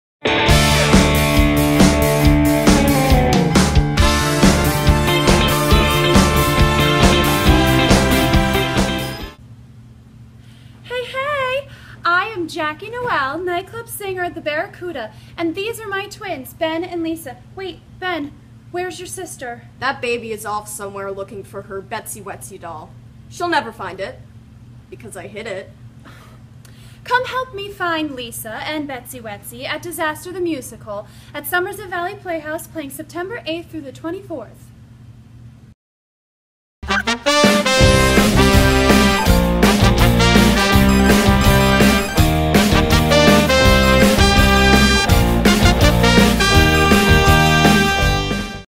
DISASTER! - Meet Jackie and Ben
It's 1979, and New York's hottest A-listers are lining up for the opening of a floating casino and discotheque.  What begins as a night of boogie fever quickly changes to panic as the ship succumbs to multiple disasters, such as earthquakes, tidal waves and infernos. As the night turns into day, everyone struggles to survive and, quite possibly, repair the love that they've lost... or at least escape the killer rats.